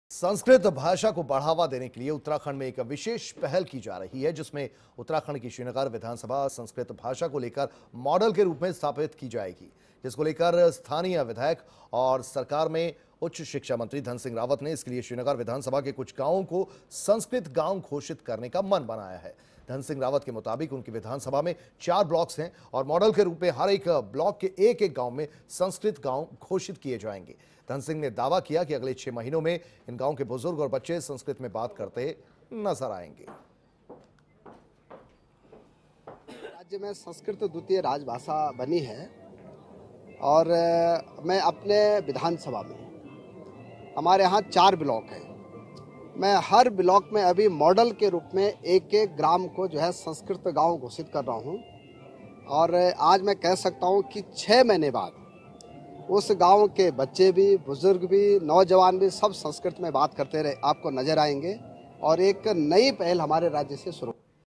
अब गांव होंगे संस्कृत के नाम, संस्कृत भाषा को लेकर धनसिंह रावत ने की पहल R9TV
उत्तराखंड की श्रीनगर विधानसभा संस्कृत भाषा को लेकर मॉडल के रूप में स्थापित करने जा रही है..जिसको लेकर स्थानीय विधायक और सरकार में उच्च शिक्षा मंत्री धनसिंह रावत ने इसके लिए श्रीनगर विधानसभा के कुछ गांवों को संस्कृत गांव  घोषित करने का मन बनाया है…